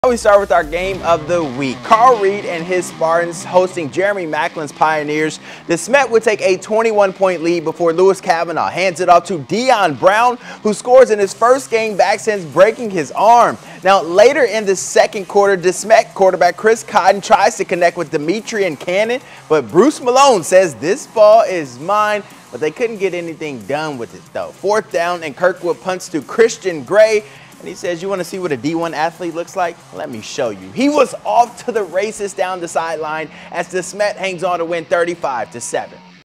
De Smet takes down Kirkwood in playoff opener
The Spartans are moving on to face rival CBC in the second round.